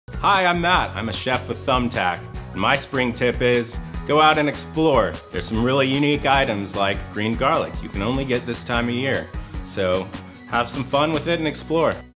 Spring Tips From Pros: Matt (Chef)
Join Thumbtack pro Matt as he shares his best spring tip. Matt is a professional chef with lots of great cooking experience.

Hi I'm Matt, I'm a chef with Thumbtack. My spring tip is go out and explore. There's some really unique items like green garlic you can only get this time of year. So, have some fun with it and explore.

Want to learn more? Check out Thumbtack to hire the right professional for your next project